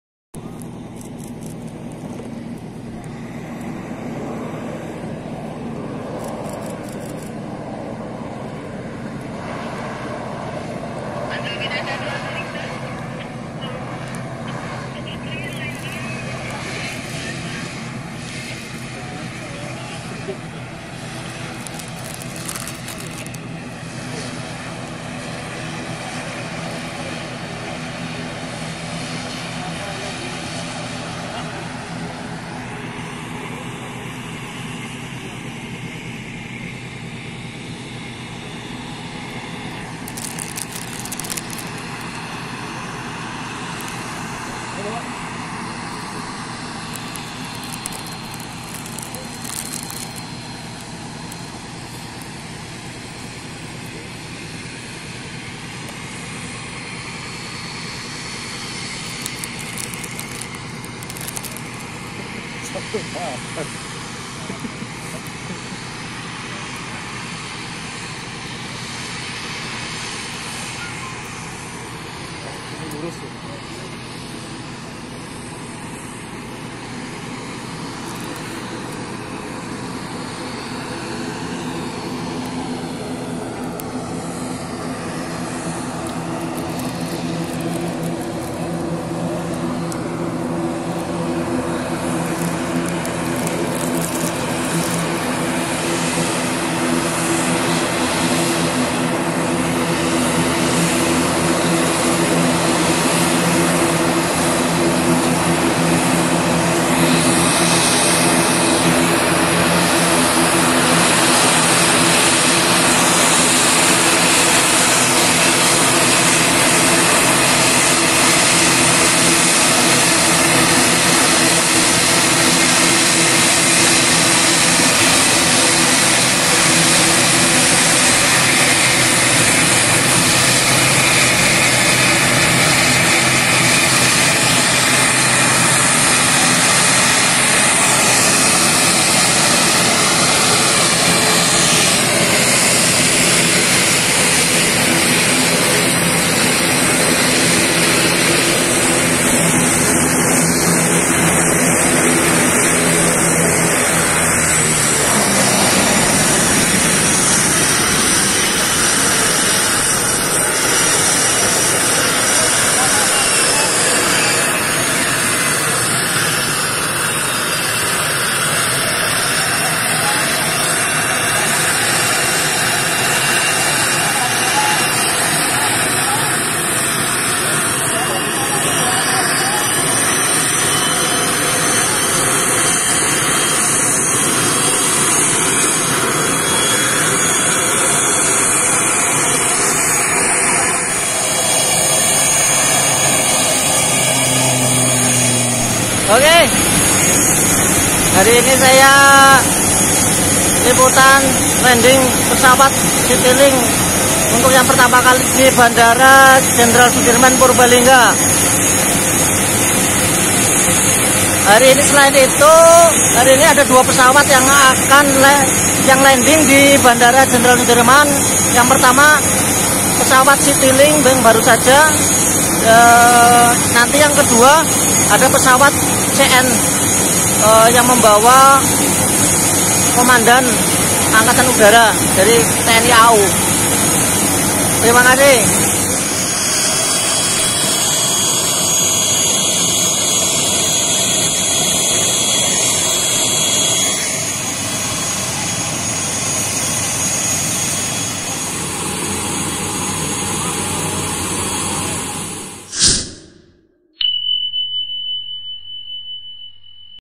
FIRST LANDING! Pesawat Citilink ATR-72 mendarat di Bandara JB Soedirman
Pesawat Komersil Citilink ATR-72 mendarat untuk yang pertama kali di Bandara Jenderal Besar Soedirmab Purbalingga, Kamis, 1 April 2021